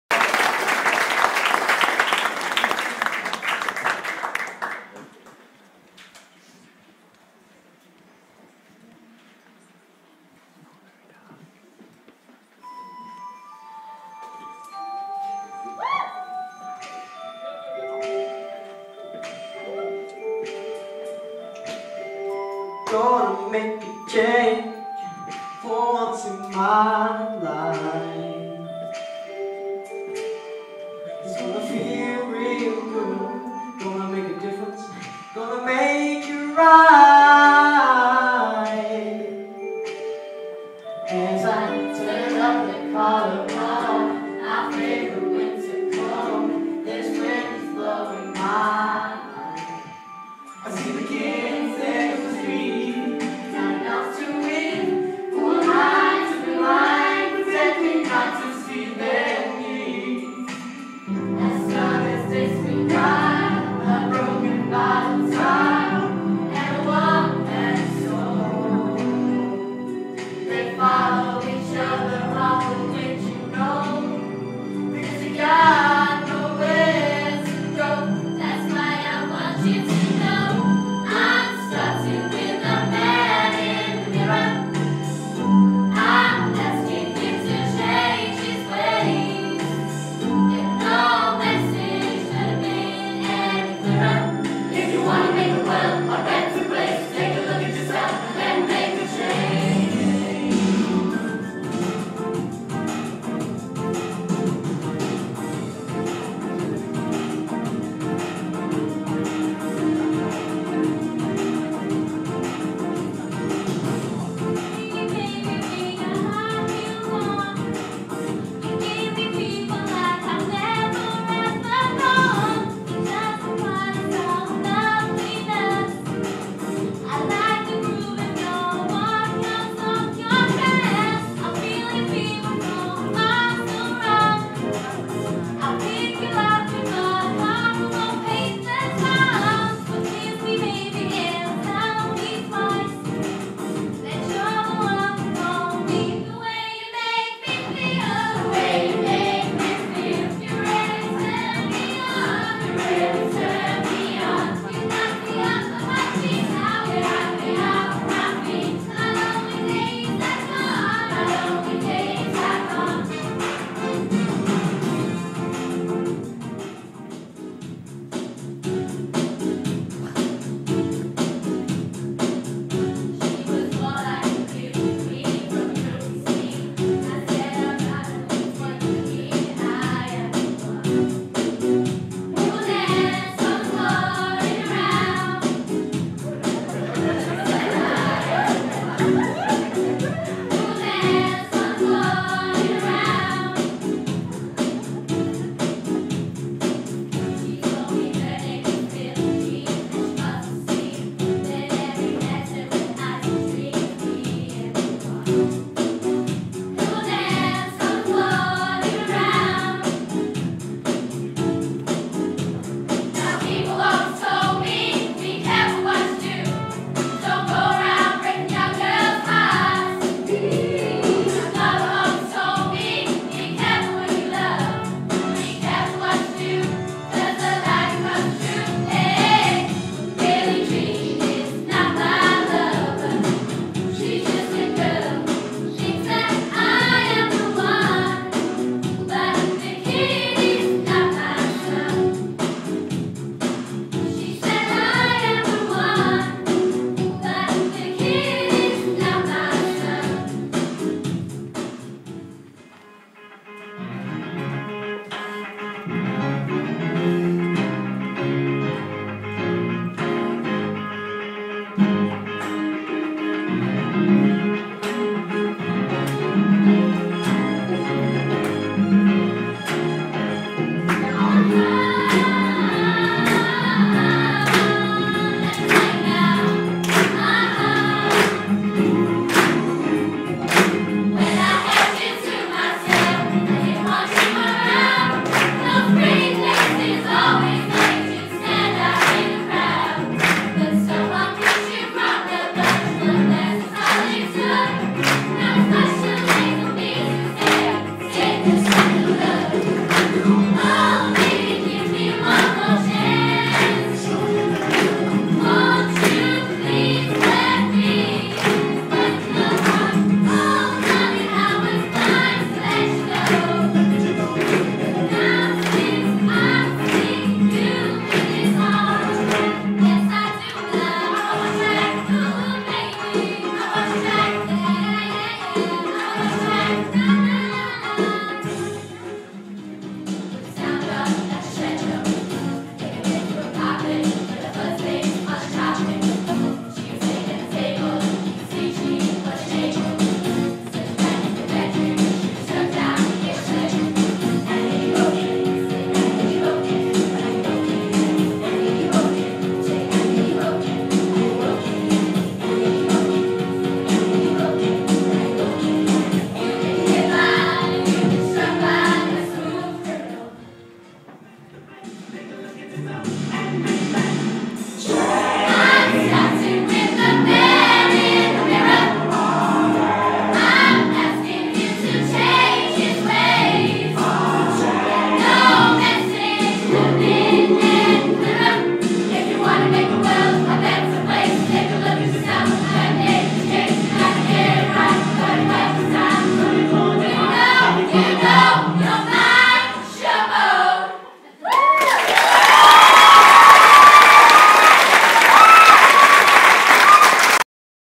Woolston High School Michael Jackson Medley
Woolston High Schools year 10 Performing arts class perfrom a short medley of Michael Jackson hits.